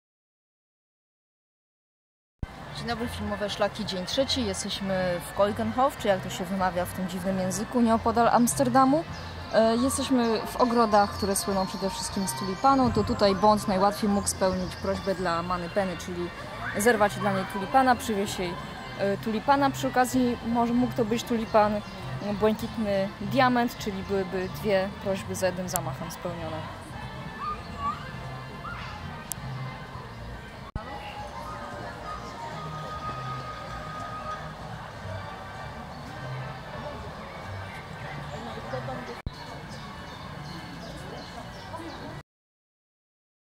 Keukenhof - wizyta w ogrodzie tulipanów
Najsłynniejszy i największy ogród tulipanów w Holandii. Ilość kwiatów to jakieś 6-7 milionów.